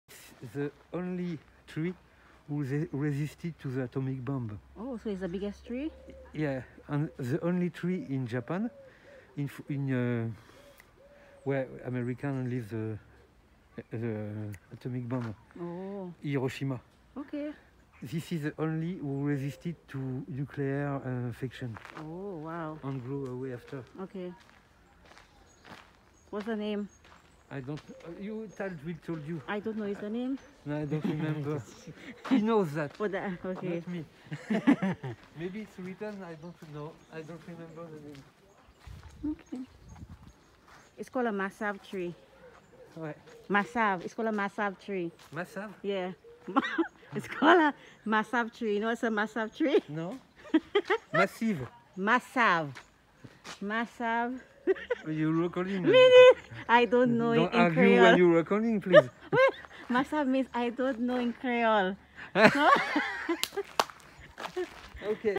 "Massav Tree" is a funny Saint Lucian joke lol
"Massav Mree" is a funny Saint Lucian joke lol

My fiance and I on vacation in the South Of France and came across this huge tree which surely has a name but he could not remember the name. However, I had to drop in my Saint Lucian joke. I had t explain to him at the end why I called it "Massav Tree" and we had a good laugh lol

There is a joke of a tourist who visited the island of Saint Lucia and was inquiring about a huge tree which is oldest tree in Saint Lucia, located at the east entrance to the Derek Walcott Square, across the street from the Cathedral of the Immaculate Conception in Castries.

Rumour has it one day, many years ago, a tourist asked a passing local "what is the name of that tree? The local answered, " massav." The word spread and soon it was known as the Massav Tree.

 Massave in Kwéyòl language means "I don't know.

Your daily dose of good vibes!!